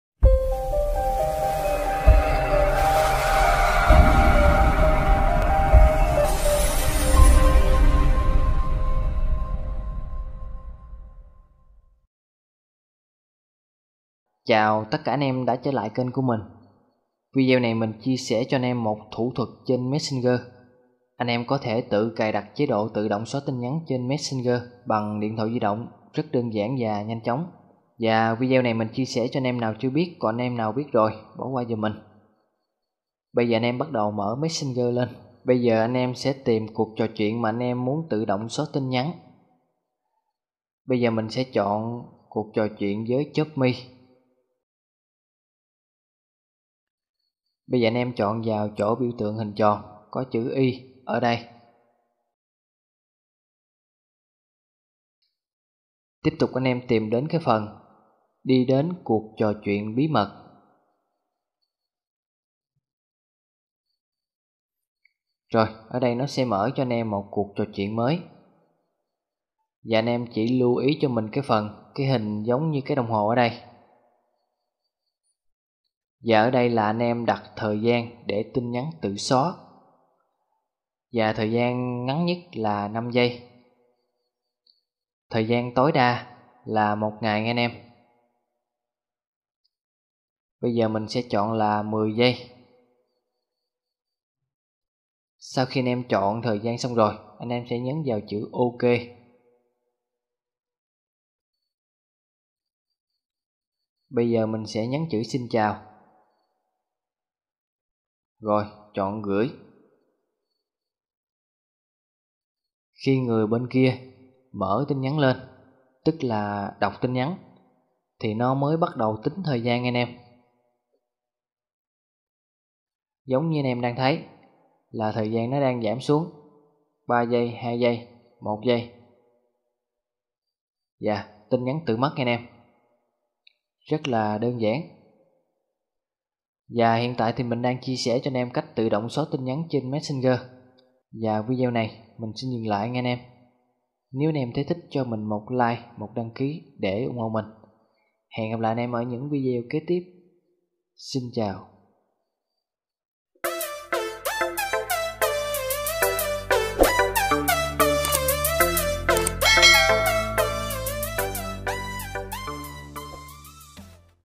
Tự động xóa tin nhắn messenger
Trong video mình chia sẻ cho anh em chế độ cài đặt trên messenger, tự động xóa được tin nhắn rất dể dàng và nhanh chống.